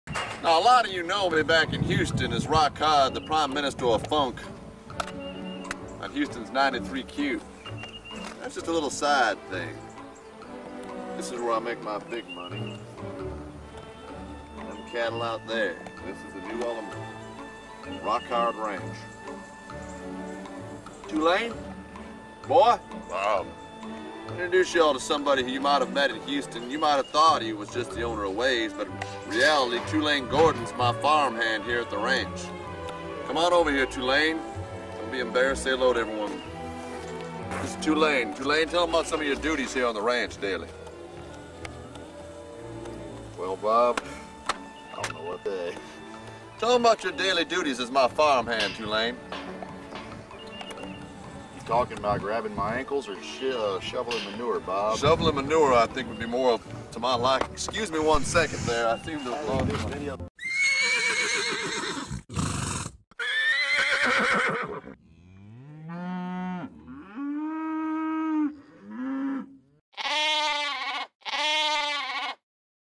Fun in New Ulm Texas with Tulane Gordon who owned/operated Waves Beach Bar in Houston
This is a short clip of me with my good friend Tulane Gordon in 1990 or 1991 at his family ranch in New Ulm, Texas.  Tulane ran the very popular Waves Beach Bsr on Westheimer in Houston during 80’s.  Great times and memories!!!  Not sure why I was wearing a dress shirt, leather chaps and a swimming trunks!!!